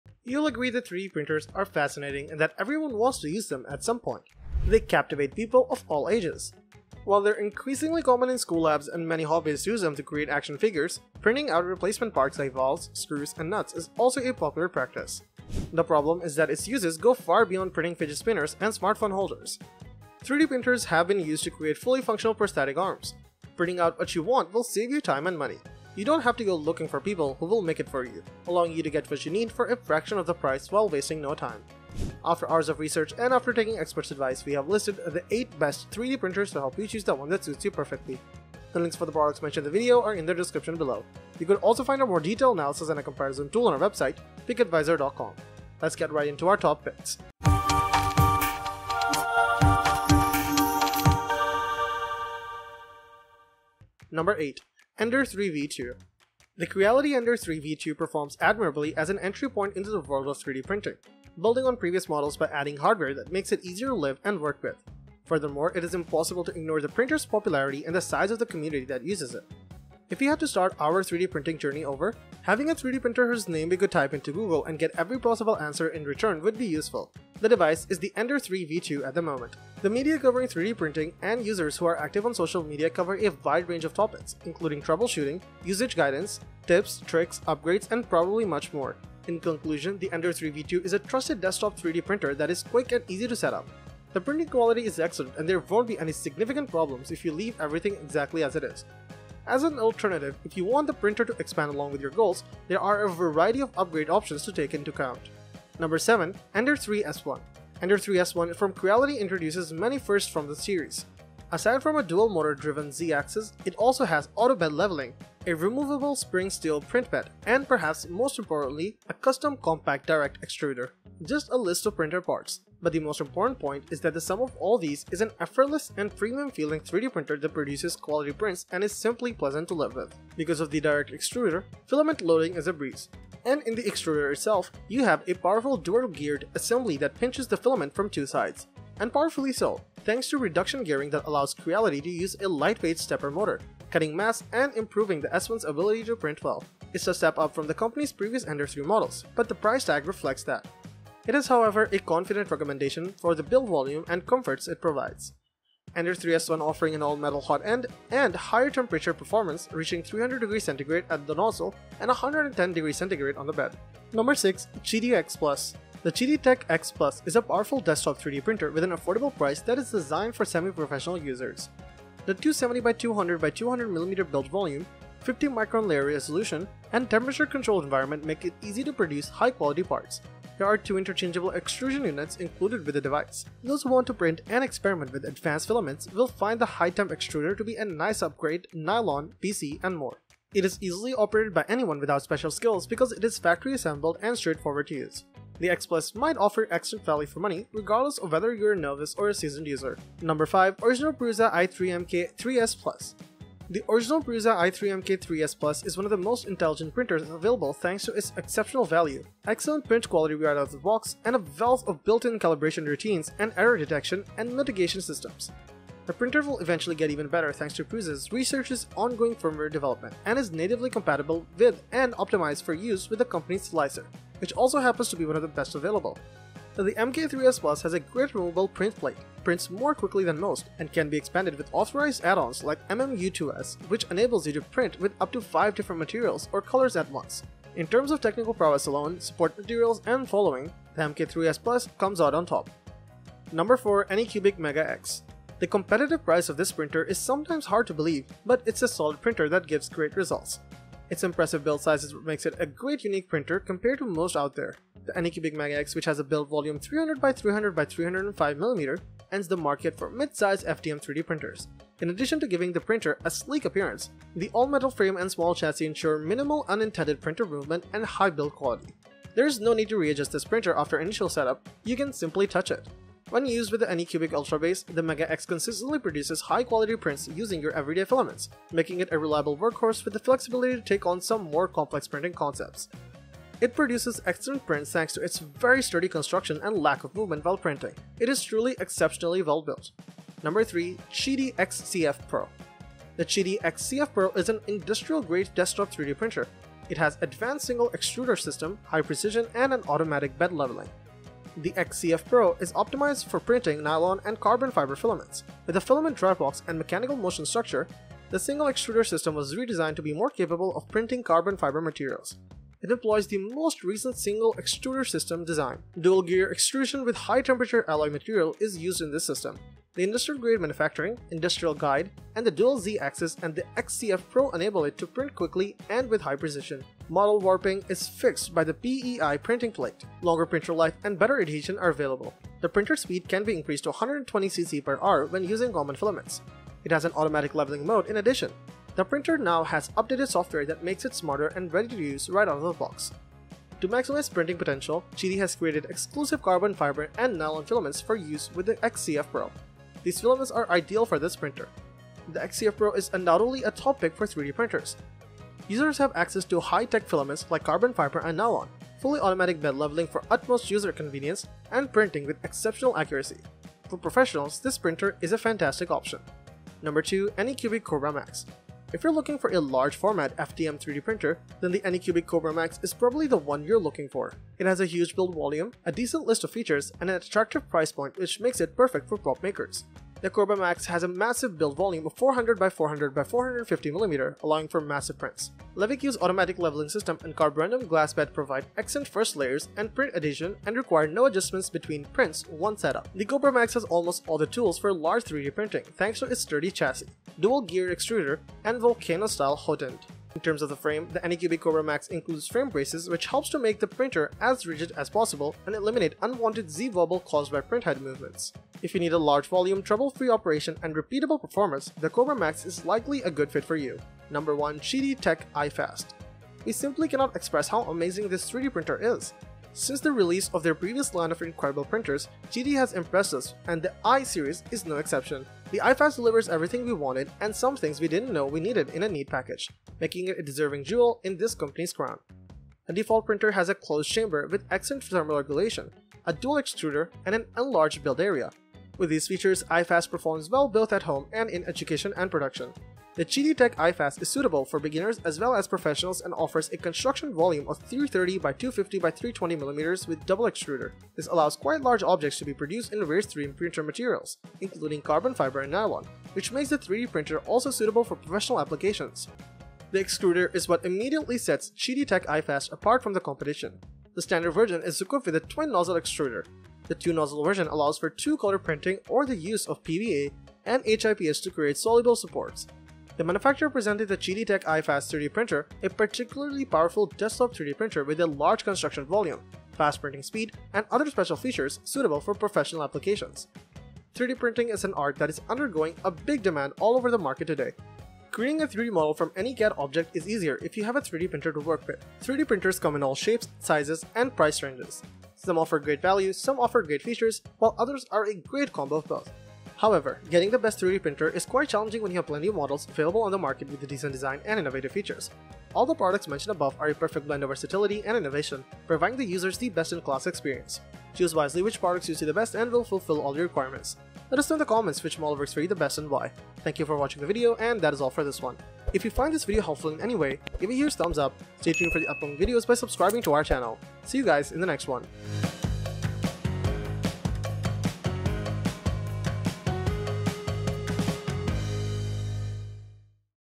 What's The Best 3D Printers (2023)? The Definitive Guide!
Best 3D Printers 2022 Shown in This Video:

8. ►Ender 3 V2
7. ►Ender 3 S1
6. ►QIDI X-Plus
5. ►Original Prusa i3 MK3S+
4. ►ANYCUBIC MEGA X
3. ►QIDI X-CF Pro
2. ►ANYCUBIC Kobra Max
1. ►QIDI Tech i-Fast

0:00​​​ -  Introduction
0:56​​​ - 8. Ender 3 V2
1:47​​​ - 7. Ender 3 S1
2:45 - 6. QIDI X-Plus
3:22​​​ - 5. Original Prusa i3 MK3S+
4:04 - 4. ANYCUBIC MEGA X
4:52 - 3. QIDI X-CF Pro
6:07​​​ - 2. ANYCUBIC Kobra Max
6:55​​​ - 1. QIDI Tech i-Fast
8:05​​​ - Conclusion

You'll agree that everyone wants to use the best 3D printers at some point because they are so fascinating. People of all ages are enthralled by them.
Printing out replacement parts like valves, screws, and nuts is a common practice, and they are becoming more and more common in school labs and among hobbyists who make action figures.
The issue is that it can be used for much more than just printing phone holders and fidget spinners. Prosthetic arms that are fully functional have been produced using 3D printers.
3D printing is an art form that is in high demand in today's market. The best 3D printer makes it simpler to convert any CAD object into a 3D model.
There are numerous types, models, and price points for 3D printers. Some have great features, and great value and others are a great combination of both. Finding the Best 3D Printers, though, can be difficult because there are so many models on the market with appealing designs and cutting-edge features. So, here's what we know about the Best 3D Printers in 2023.